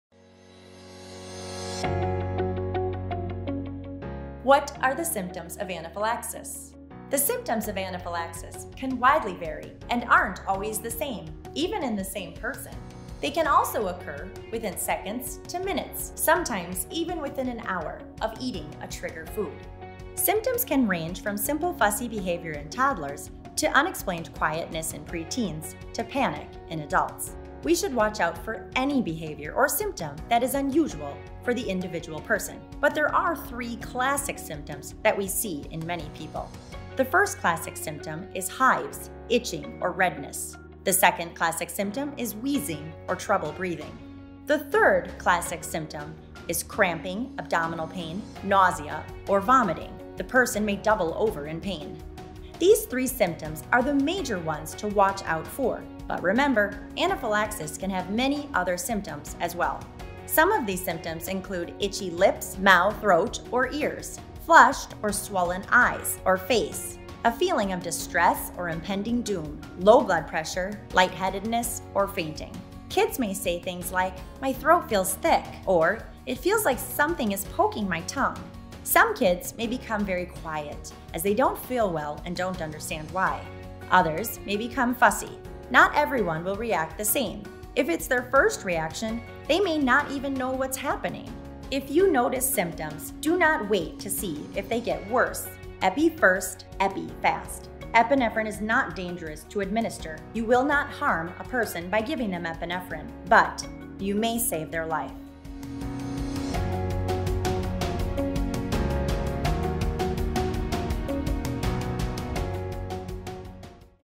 Signs and Symptoms of Anaphylaxis
The symptoms of anaphylaxis can widely vary and aren't always the same, even in the same person. The three classic symptoms include 1) hives, itching or redness, 2) wheezing or trouble breathing, and 3) cramping, abdominal pain, nausea, or vomiting. While these are the major symptoms to watch out for, individuals should look out for any unusual behaviors or symptoms and be ready to administer epinephrine.